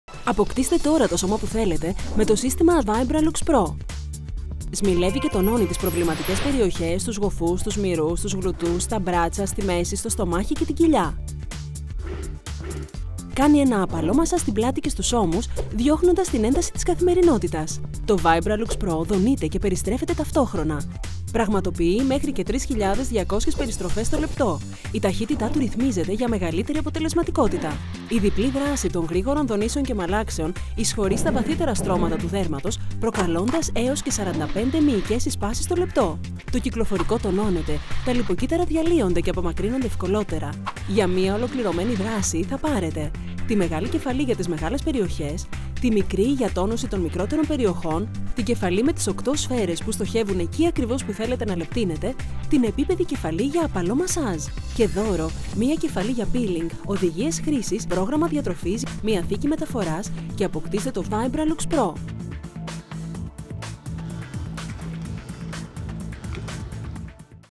Vibraluxe Pro - Συσκευή μασάζ
Η συσκευή Vibraluxe Pro δονείται και περιστρέφεται ταυτόχρονα έως και 3.200 στροφές ανά λεπτό για να χαρίσει ένα βαθύ επαγγελματικό μασάζ που καίει λίπος, τονώνει το κυκλοφορικό και συσφίγγει ολόκληρο το σώμα.

Ιδανική επίσης για ανακούφιση από τους πόνους και την ένταση της καθημερινότητας.
Με ρύθμιση ταχύτητας ανάλογα για τις προσωπικές σας ανάγκες.
Με δονήσεις που φτάνουν στα βαθύτερα στρώματα της επιδερμίδας και δημιουργούν 45 συσπάσεις ανά λεπτό, σαν να σηκώνετε βάρη ή να κάνετε καθίσματα.
Αποτελεσματική δράση ύστερα από 15 μέρες χρήσης σύμφωνα με κλινικές μελέτες.

Διαθέτει 6 διαφορετικές κεφαλές-εξαρτήματα:
-Την απαλή κεφαλή για ένα χαλαρωτικό μασάζ.
-Τη μεγάλη κυματοειδή κεφαλή που τονώνει και σμιλεύει.
-Τη μικρή κυματοειδή κεφαλή για τις ευαίσθητες περιοχές.
-Την έντονη ανάγλυφη κεφαλή με πολλούς "κόμπους" για αδυνάτισμα και σύσφιγξη.
-Την ελαφρόπετρα που λειαίνει το σκληρό δέρμα σε αγκώνες και πέλματα.
-Το προστατευτικό κάλυμμα για πιο απαλό μασάζ.

Χρησιμοποιήστε το Vibraluxe Pro στους μηρούς, στους γοφούς, στην κοιλιά, στην πλάτη, στα χέρια και στα πόδια.
Αρκούν λίγα λεπτά χρήσης την ημέρα για να δείτε ξανά σφιχτή και νεανική επιδερμίδα.